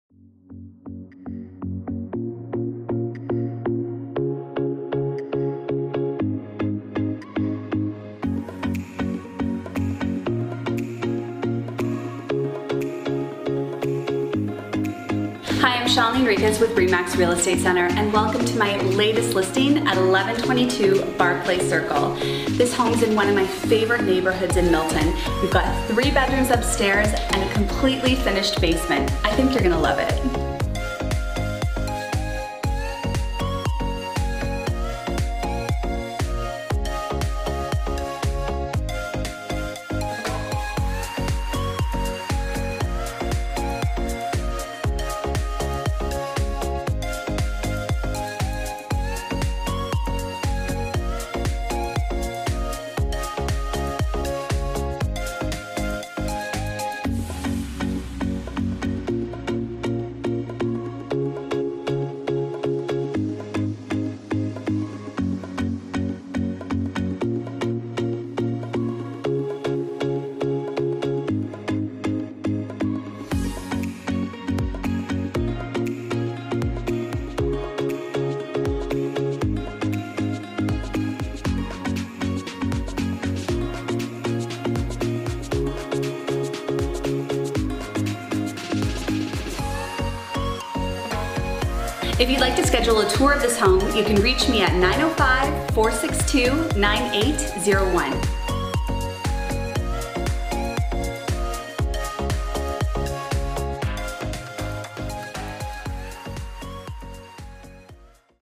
1122 Barclay Circle | SOLD | Townhome For Sale in Milton
1122 Barclay Circle this beautiful Mattamy Bryant Model townhome is ready for you to move right in.

Located in the sought after Beaty neighbourhood, the neighbours all share a real sense of community. Here you'll find kids playing outside, neighbours that are quick to greet you with a smile, and that really look out for each other. It's a lovely place to call home.

This popular open floor plan gives you a defined dining room and family room space and is overlooked by the spacious kitchen.

Upstairs you'll find 3 generous sized bedrooms. The master bedroom has its own walk-in closet , and there's a semi-ensuite bathroom.

The basement is finished, open concept and makes the perfect spot for movie night. There's a 3pc washroom as well.

1122 Barclay is a lovely home in a charming neighbourhood and is ready for its new owners.

You'll truly love living here.